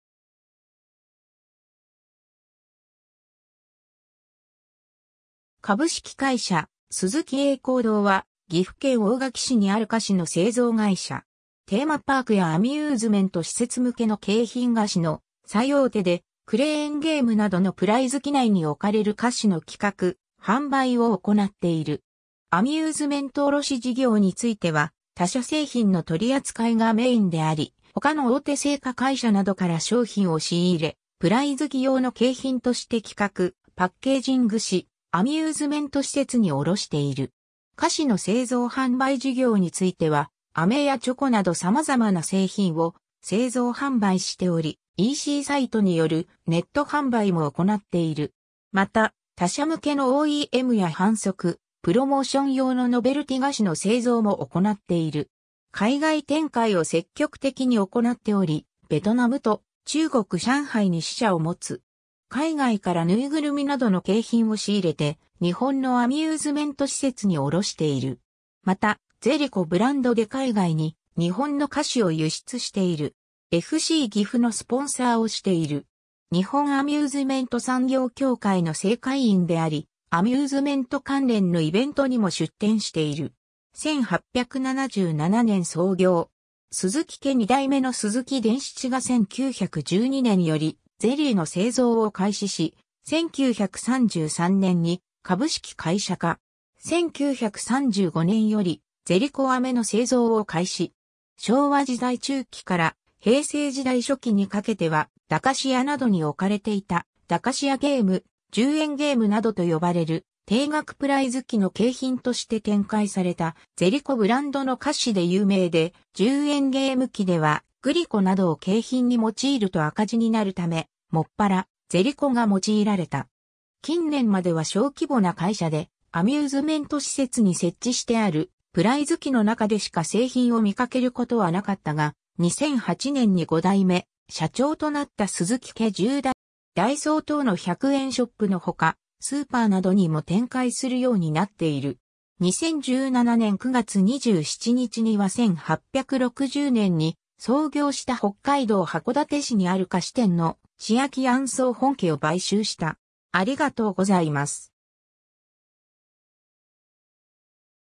鈴木栄光堂
株式会社鈴木栄光堂（すずきえいこうどう）は、岐阜県大垣市にある菓子の製造会社。
 テーマパークやアミューズメント施設向けの景品菓子の最大手で、クレーンゲームなどのプライズ機内に置かれる菓子の企画・販売を行なっている。
アミューズメント卸事業（AM事業）については他社製品の取扱いがメインであり、他の大手製菓会社などから商品を仕入れ、プライズ機用の景品として企画・パッケージングし（巨大なパッケージのチロルチョコなど）、アミューズメント施設に卸している。
 菓子の製造販売（一般流通）事業については、アメやチョコなど様々な製品を製造販売しており、ECサイトによるネット販売も行っている。
また、他社向けのOEMや販促・プロモーション用のノベルティ菓子の製造も行っている。
 海外展開を積極的に行っており、ベトナムと中国上海に支社を持つ。
海外からぬいぐるみなどの景品を仕入れて日本のアミューズメント施設に卸している。
また、「Zelico」ブランドで海外に日本の菓子を輸出している。
 FC岐阜のスポンサーをしている。
日本アミューズメント産業協会の正会員であり、アミューズメント関連のイベントにも出店している。
 1877年創業。
鈴木家2代目の鈴木伝七（初代社長）が1912年よりゼリーの製造を開始し、1933年に株式会社化。
1935年より「ゼリコ飴」（ゼリコ キヤンデイ）の製造を開始。
 昭和時代中期から平成時代初期にかけては、駄菓子屋などに置かれていた「駄菓子屋ゲーム」「10円ゲーム」などと呼ばれる低額プライズ機の景品として展開された「ゼリコ」ブランドの菓子で有名で、10円ゲーム機では「グリコ」などを景品に用いると赤字になるため、もっぱら「ゼリコ」が用いられた（そのため「グリコのパチモン」と誤解されがちだが、「グリコ」の発売が1919年であるため、1912年発売の「ゼリコ」の方が先である。ただし「ゼリコ」ブランドがいつから用いられているのかは不明）。
 近年までは小規模な会社で、アミューズメント施設に設置してあるプライズ機の中でしか製品を見かけることはなかったが、2008年に5代目社長となった鈴木家10代目の鈴木伝が積極的なM&Aを展開し、鈴木伝が入社した2002年当時は5人だった社員を、2019年時点でグループ全体で300人にまで急拡大。
ダイソー等の100円ショップの他、スーパーなどにも展開するようになっている。
 2017年9月27日には1860年に創業した北海道函館市にある菓子店の千秋庵総本家を買収した。